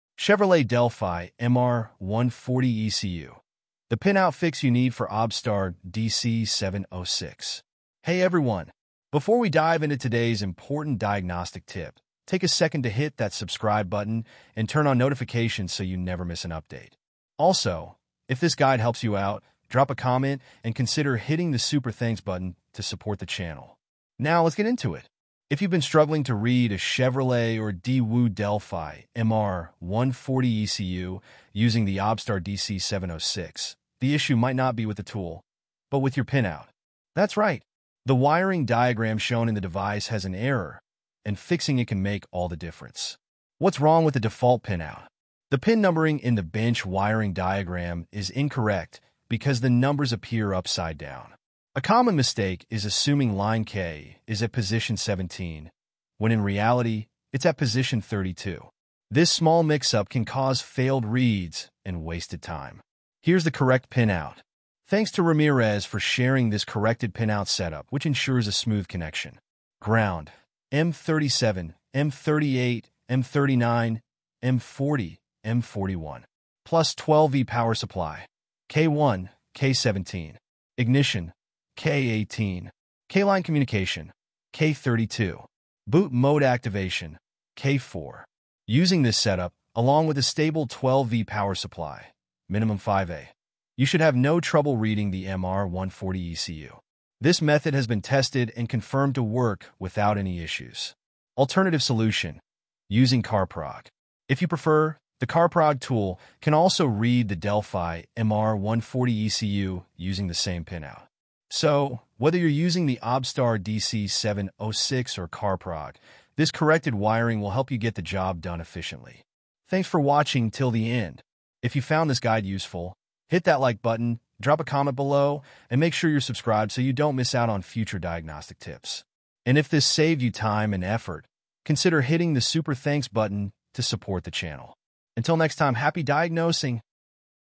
Chevrolet Delphi MR140 ECU: The Pinout Fix You Need for OBDSTAR DC706
🔧 Chevrolet Delphi MR140 ECU Pinout Fix – OBDSTAR DC706 Users Must Watch! 🔧

Struggling to read the Chevrolet or Daewoo Delphi MR140 ECU with your OBDSTAR DC706? The problem might be with the incorrect pinout in the default wiring diagram! In this video, we’ll show you the correct wiring setup to ensure a successful read every time.

🚗 Key Fixes Covered:
✅ Correct MR140 ECU Pinout (Line K is actually at position 32, NOT 17!)
✅ Proper grounding and power supply connections
✅ How to activate Boot Mode for reading
✅ Alternative method using CarProg

📌 Correct Pinout for Delphi MR140 ECU:
⚡ Ground: M37, M38, M39, M40, M41
⚡ +12V Power Supply: K1, K17
⚡ Ignition: K18
⚡ K-Line Communication: K32
⚡ Boot Mode Activation: K4

🔥 Stop wasting time on failed reads! Use this corrected setup with a 12V 5A power supply and get your MR140 ECU working flawlessly with OBDSTAR DC706 or CarProg.